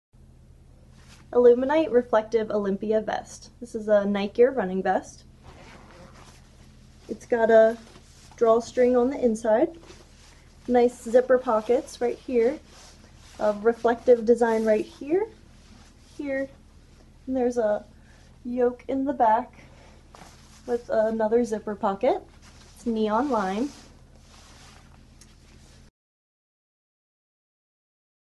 illumiNITE Reflective Olympia Vest 52101
Reflective Dot pattern is on the front and back yoke, on the sides and the back waist. Reflective logos are on the front. It has 2 zipper front pockets, 2 inner pockets, and a large Zipper pocket in the back. There is a breathable yoke in the back, and an inner drawstring waist. Great Hi Viz color of Neon Lime. Illuminite Reflective Olympia Vest number 52101.